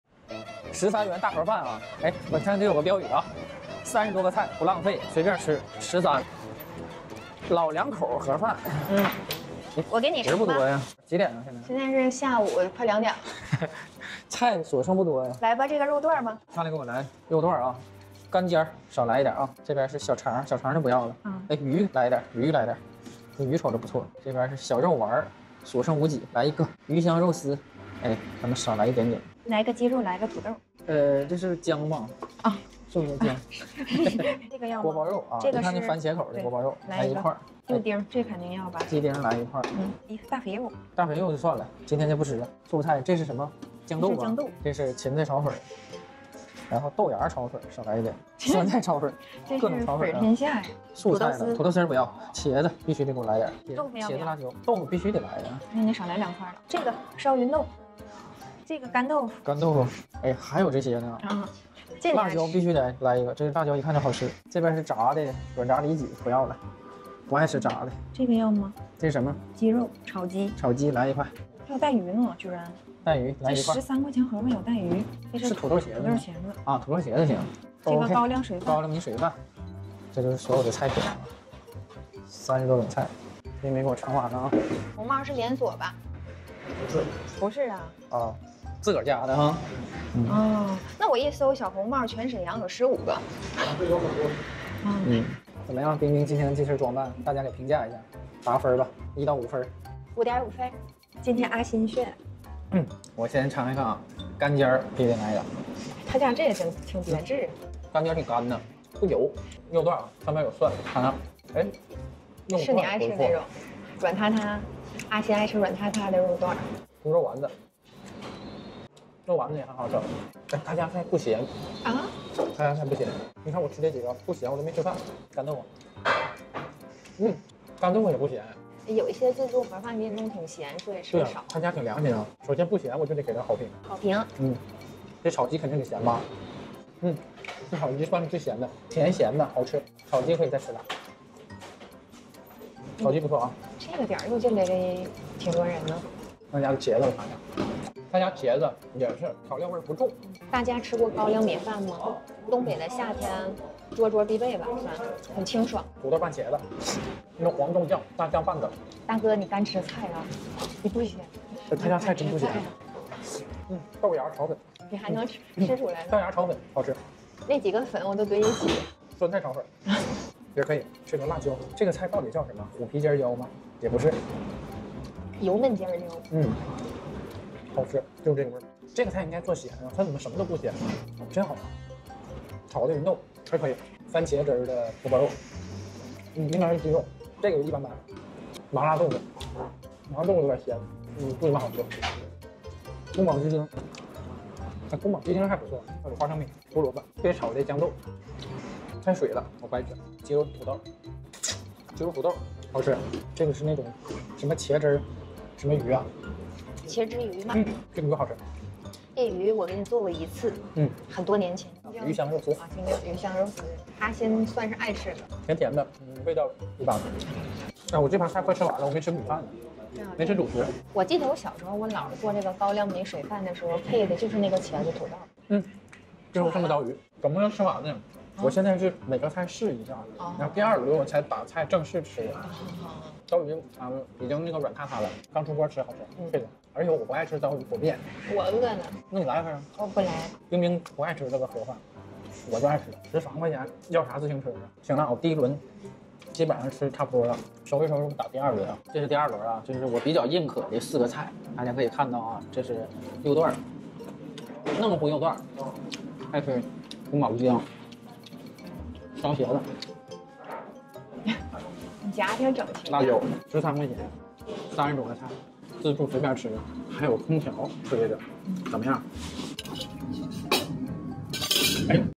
13元自助盒飯30多個菜炸帶魚隨便吃-蘇家屯小紅帽盒飯
025-13元自助盒飯30多個菜炸帶魚隨便吃
13元自助盒飯30多個菜炸帶魚隨便吃-蘇家屯小紅帽盒飯